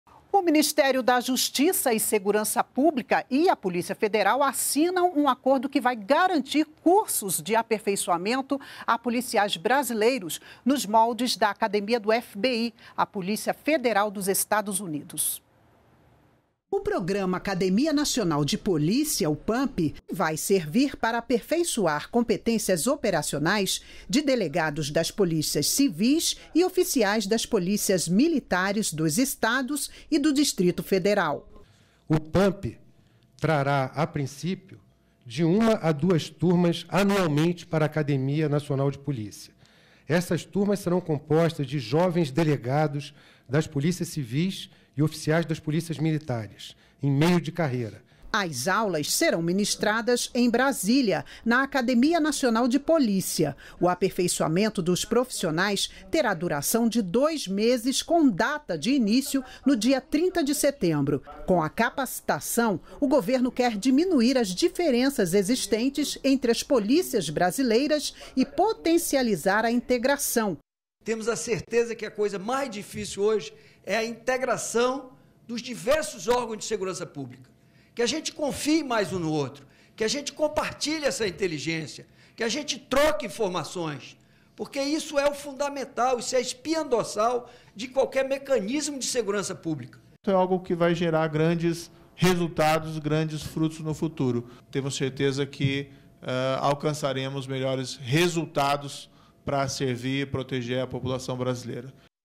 Brasil vai capacitar policiais nos moldes do FBI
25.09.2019 - O Ministério da Justiça e Segurança Pública e a Polícia Federal assinam um acordo que vai garantir cursos de aperfeiçoamento a policiais brasileiros, nos moldes da academia do FBI, a polícia federal dos Estados Unidos.